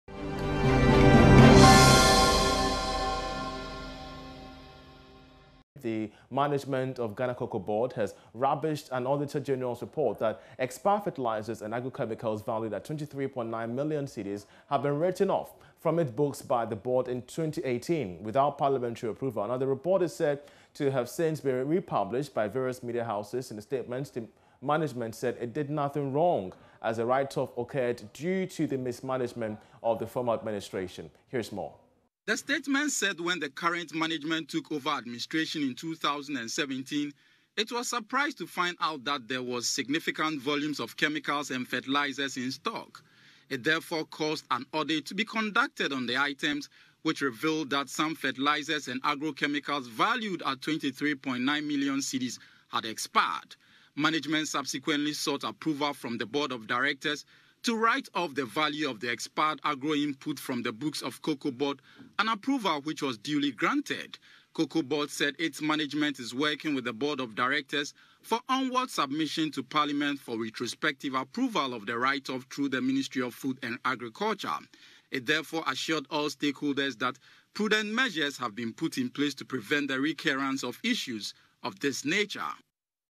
COCOBOD discounts not seeking parliamentary approval over expired fertilizers (25-8-21)
Auditor General’s Report: COCOBOD discounts not seeking parliamentary approval over expired fertilizers (25-8-21)